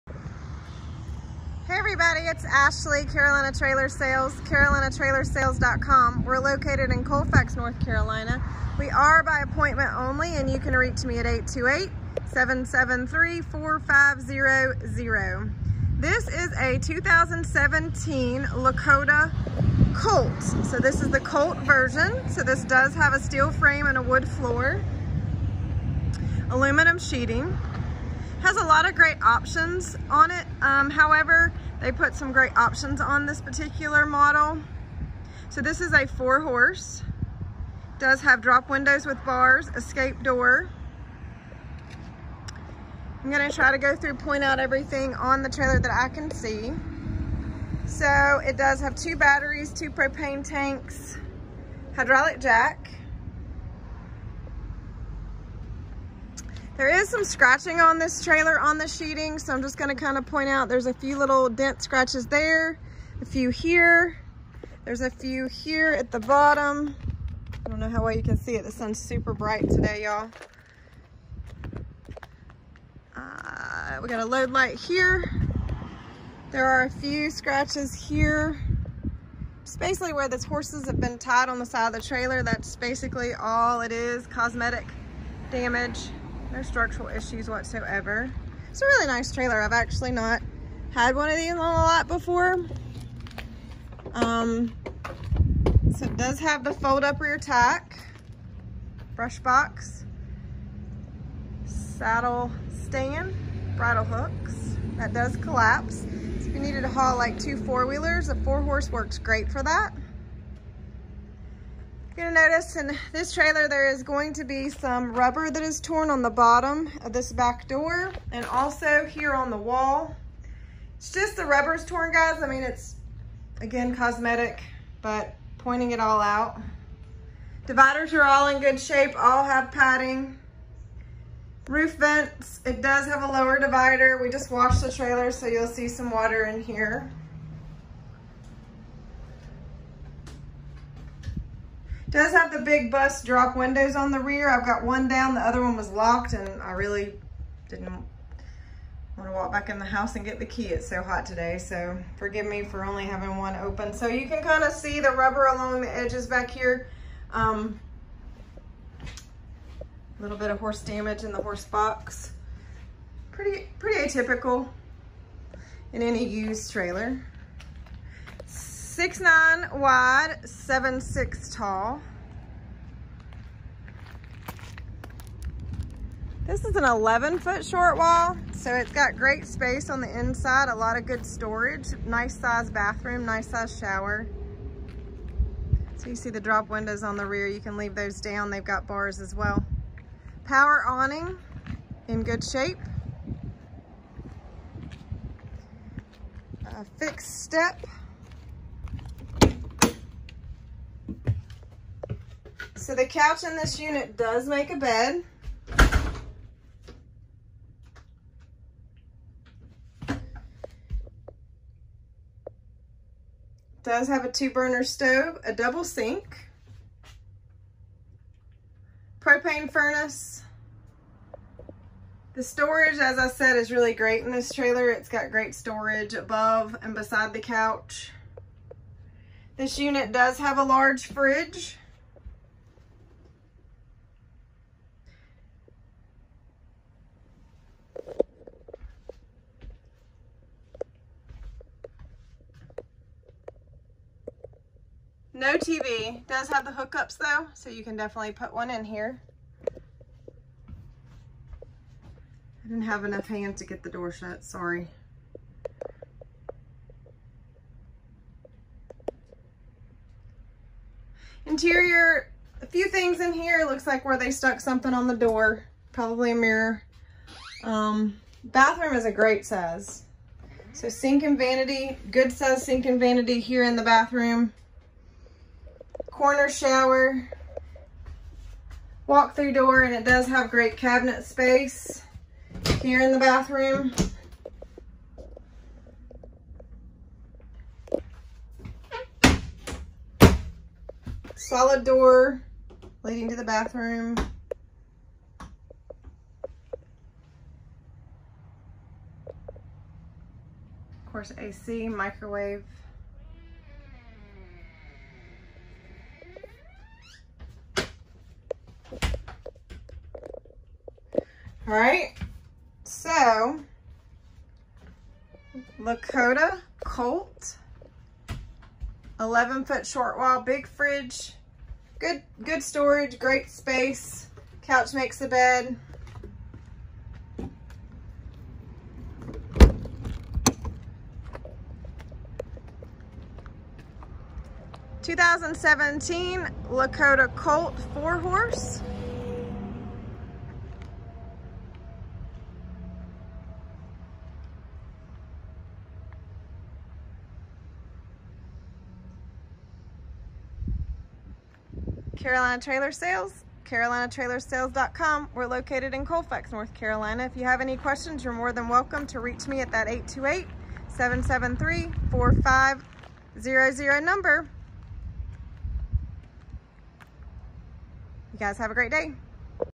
SOLD 💥 Pre-Owned 2017 Lakota 4 Horse Trailer, 11' FULLY LQ, Hydraulic Jack, Model: AC411 🐎🐎🐎🐎
2017 Lakota AC411 4 horse 11ft full LQ, big fridge, rear kitchen with double sinks, gas stove, multiple closets, cabinets, couch makes a bed, Walk through door, big bathroom with sink and vanity, corner large shower, power awning, hydraulic jack, rear fold up tack, drop windows with bars, two large bud drop windows on the rear, escape door, lower divider, some cosmetic damage on outside sheeting in a few places, some back wall rubber torn at the bottom of rear wall, all cosmetic!